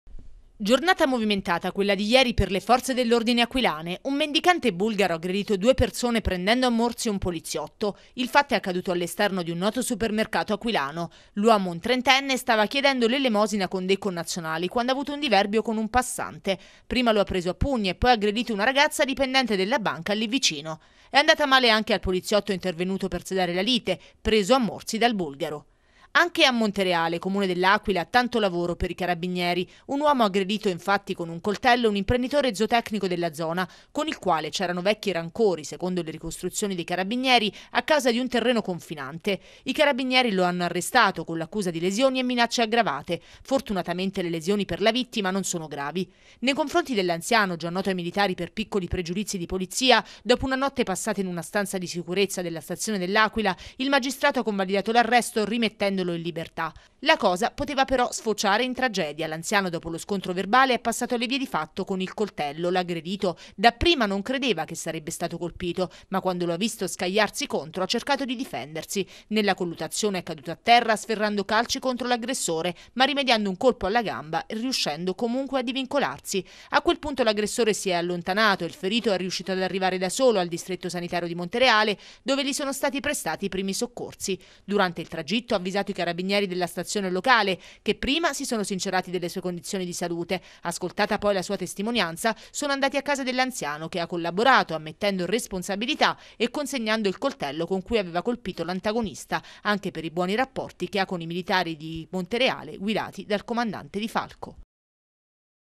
AQ Cronaca L'Aquila aggressioni
Ha aggredito due persone ed un agente di polizia, intervenuto per riportare la calma, un mendicante di origine bulgara. IL fatto è avvenuto davanti ad un supermercato.